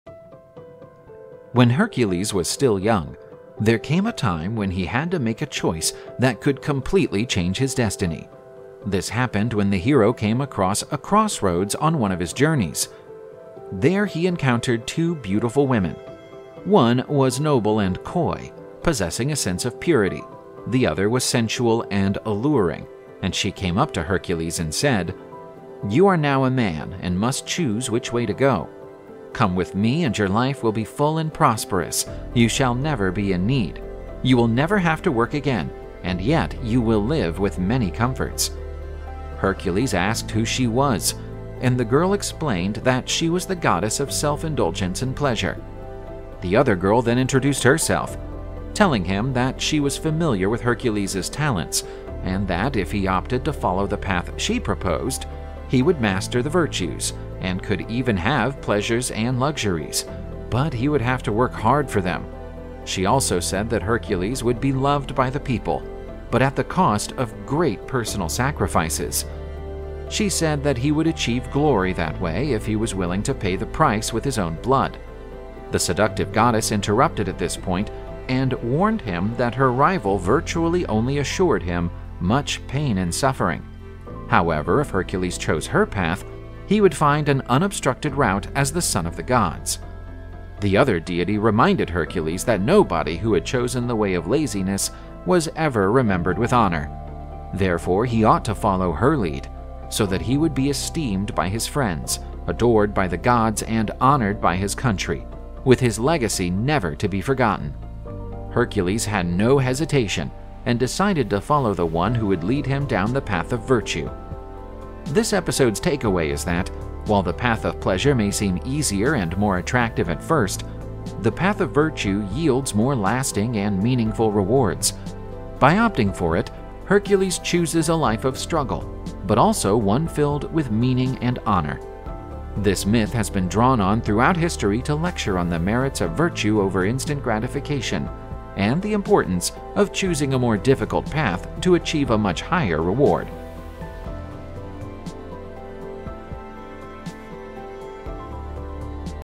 Pleasures or Glory: The Choice of Hercules
In this famous allegory from Greek mythology, Hercules must choose between two paths: the difficult and honorable path of Virtue, and the easy and pleasurable path of Vice. Hercules' moral choice portrays the hero's journey towards virtue.

See U in History

Color: Arthur Russo